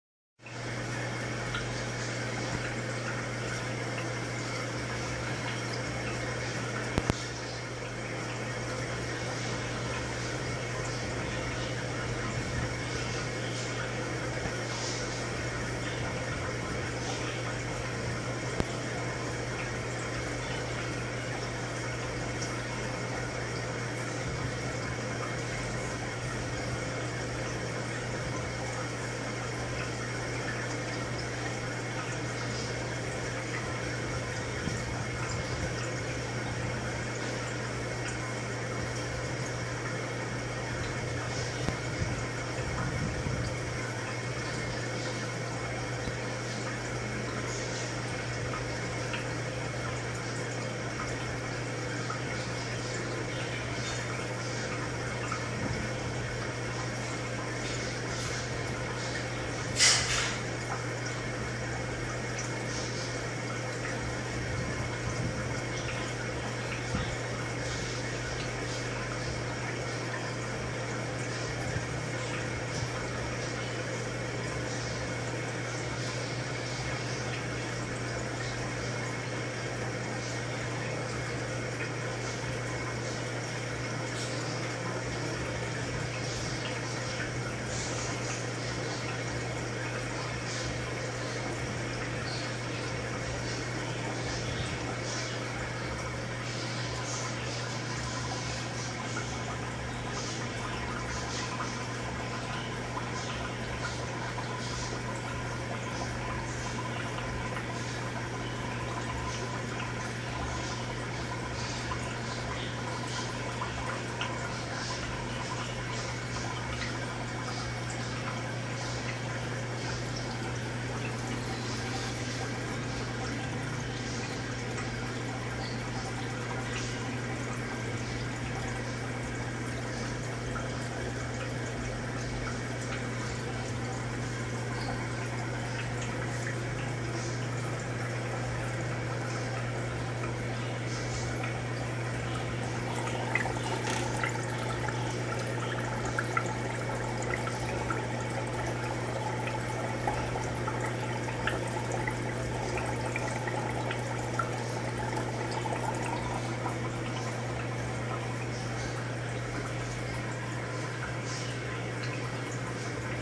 Aquário Dividido Oscar( Apaiari ) com Tambaqui
Aquário com Tambaqui e Oscar ( Apaiari ).
O Aquario foi dividido ao meio com um vidro, devido às brigas extremas entre os 2 peixes.

Aquarium with Tambaqui and Oscar (Apaiari).
The Aquarius was divided in half with a glass due to the extreme fights between the two fish.

コロソマとオスカー（Apaiari）で水族館。
アクエリアスは、二匹の魚の間の極端な戦いのためにガラスを半分に分けました。